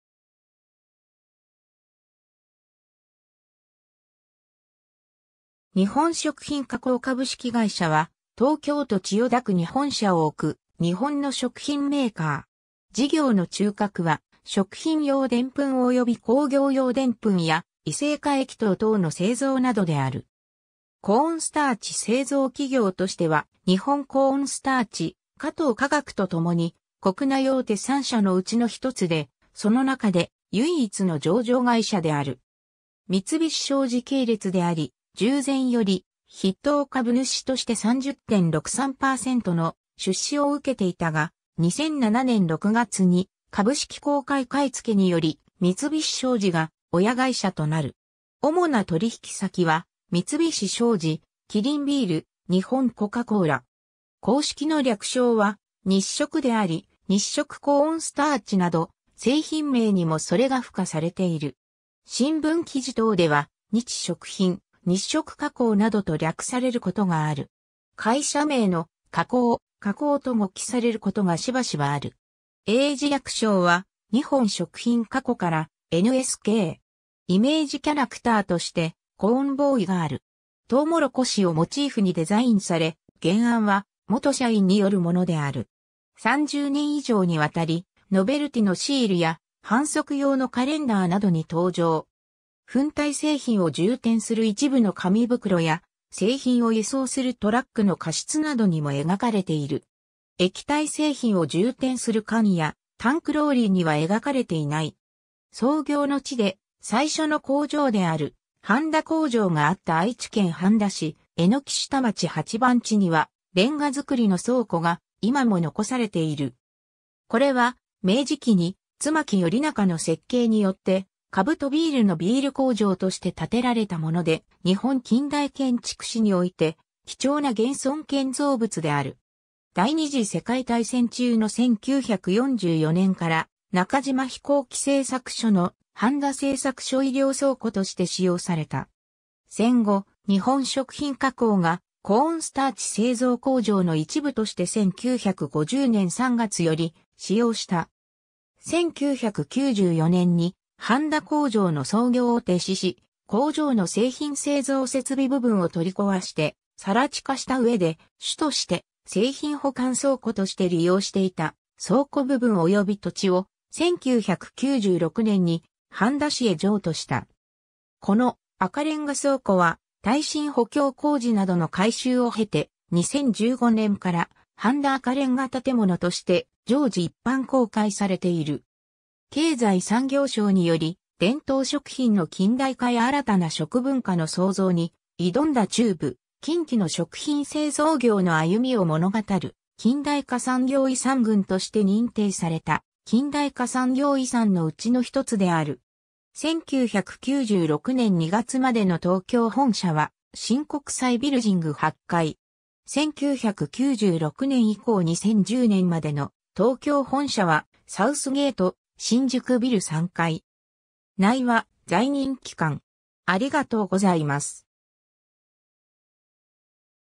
日本食品化工
日本食品化工株式会社（にほんしょくひんかこう、英: Nihon Shokuhin Kako Co., Ltd.）は、東京都千代田区に本社を置く、日本の食品メーカー。
 事業の中核は、食品用デンプンおよび工業用デンプンや異性化液糖等の製造などである。
コーンスターチ製造企業としては日本コーンスターチ、加藤化学とともに国内大手3社のうちの一つで、その中で唯一の上場会社（東京証券取引所第二部上場）である。
三菱商事系列であり、従前より筆頭株主として30.63%の出資を受けていたが、2007年（平成19年）6月に株式公開買付けにより三菱商事が親会社となる。
主な取引先は、三菱商事、キリンビール、日本コカ・コーラ。
 公式の略称は「日食」であり、「日食コーンスターチ」など製品名にもそれが付加されている。
新聞記事等では「日食品」「日食化工」などと略されることがある。
会社名の「化工」を「加工」と誤記されることがしばしばある。
英字略称は、Nihon Shokuhin Kakoから「NSK」。
 イメージキャラクターとして、「コーンボーイ（CORN BOY）」がある。
とうもろこしをモチーフにデザインされ、原案は元社員によるものである。
30年以上にわたり、ノベルティのシールや販促用のカレンダーなどに登場。
粉体製品を充填する一部の紙袋や、製品を輸送するトラックの荷室などにも描かれている。
液体製品を充填する缶や タンクローリーには描かれていない。
 創業の地で、最初の工場である半田工場があった愛知県半田市榎下町8番地には、煉瓦造りの倉庫が今も遺されている。
これは、明治期に妻木頼黄の設計によって、カブトビールのビール工場として建てられたもので、日本近代建築史において貴重な現存建造物である。
第二次世界大戦中の1944年（昭和19年）から中島飛行機製作所の半田製作所衣糧倉庫として使用された。
戦後、日本食品化工がコーンスターチ製造工場の一部として1950年（昭和25年）3月より使用した。
1994年（平成6年）に半田工場の操業を停止し、工場の製品製造設備部分を取り壊して更地化した上で、主として製品保管倉庫として利用していた倉庫部分および土地を、1996年（平成8年）に半田市へ譲渡した。
この赤レンガ倉庫は、耐震補強工事などの改修を経て、2015年（平成27年）から「半田赤レンガ建物」として常時一般公開されている。
経済産業省により「伝統食品の近代化や新たな食文化の創造に挑んだ中部・近畿の食品製造業の歩みを物語る近代化産業遺産群」として認定された近代化産業遺産のうちの一つである。
 1996年2月までの東京本社は、新国際ビルヂング8階（東京都千代田区丸の内3-4-1）。
 1996年以降 2010年までの東京本社は、サウスゲート新宿ビル3階（東京都渋谷区千駄ヶ谷5-33-8）。
 〔 〕内は在任期間